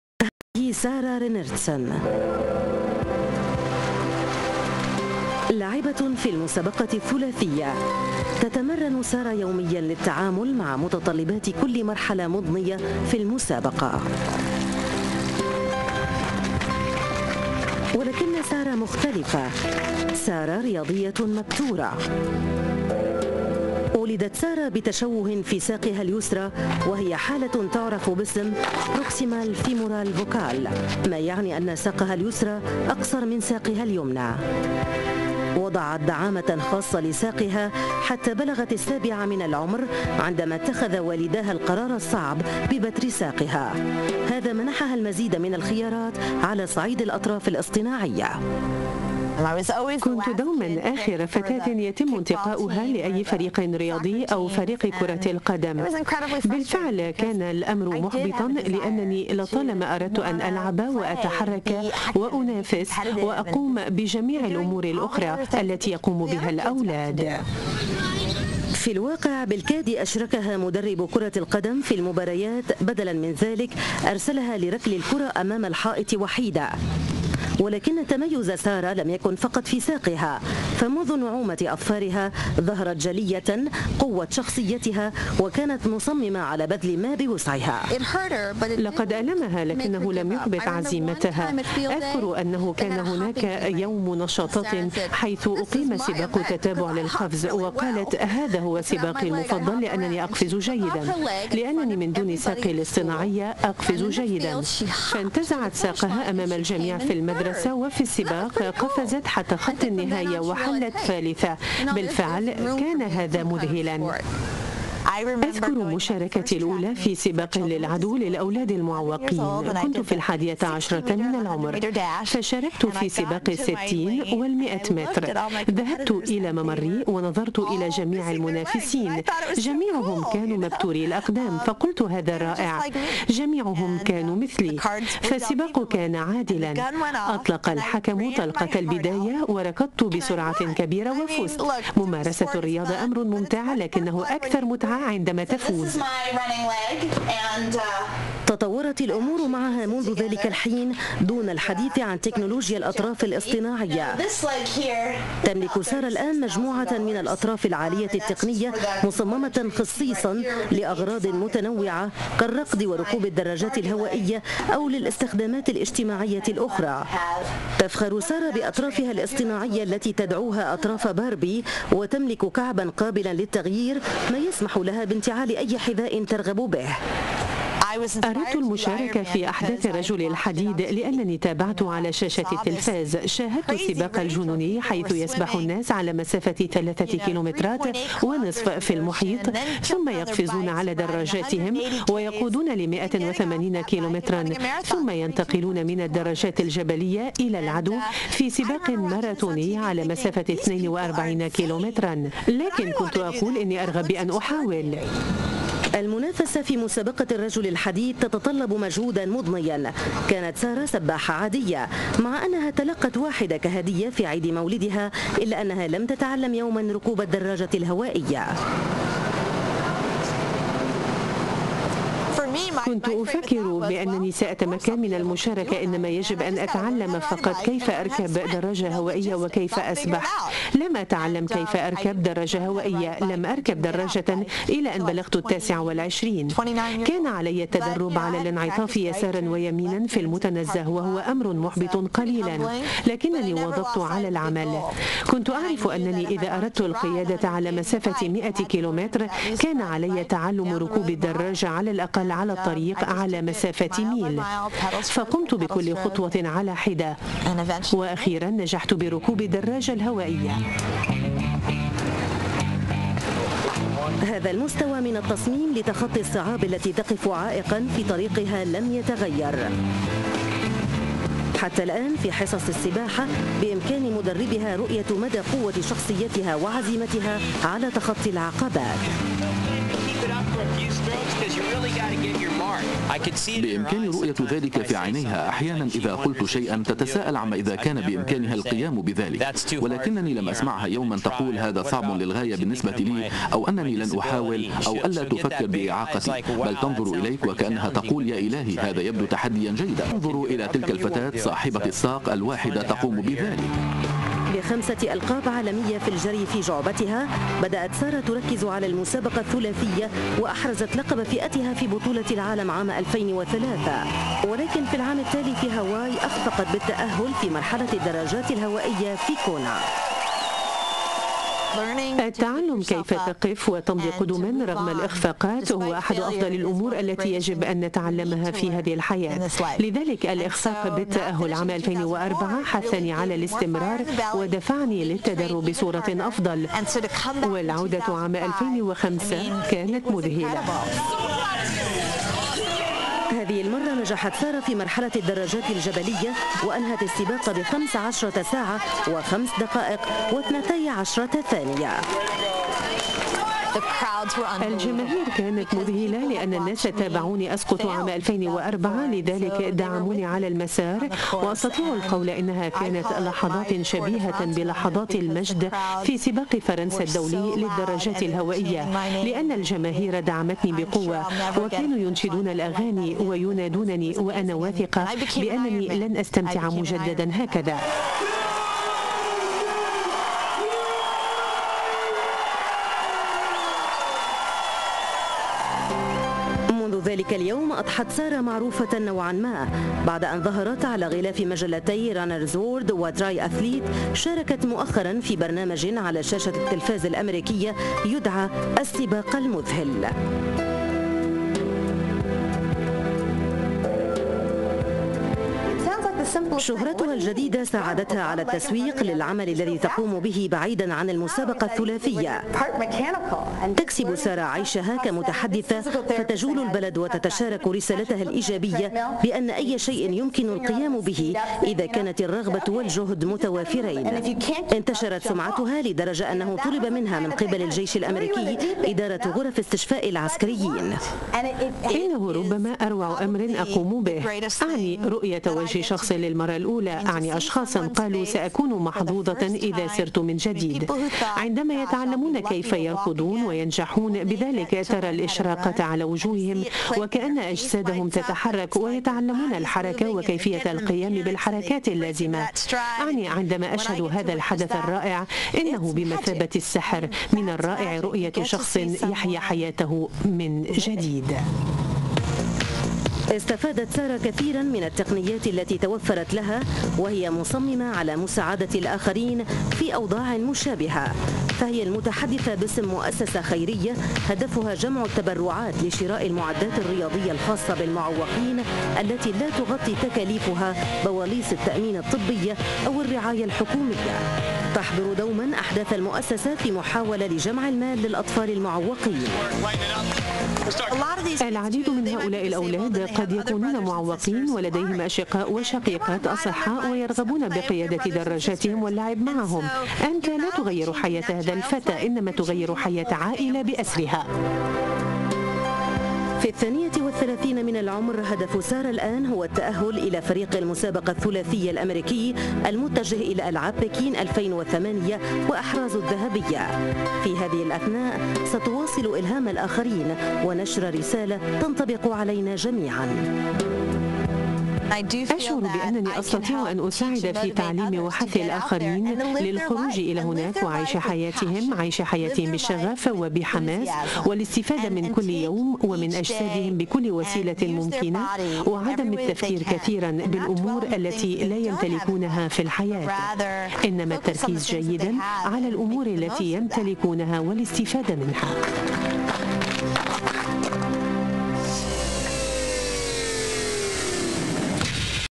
سارة رينرتسن - قصة كفاح فتاة | Sarah Reinertsen
تقرير منقول عن بطلة الالعاب الثلاثية ( الجري - السباحة - ركوب الدراجة ) سارة رينرتسن
 - منقول عن قناة الجزية الرياضية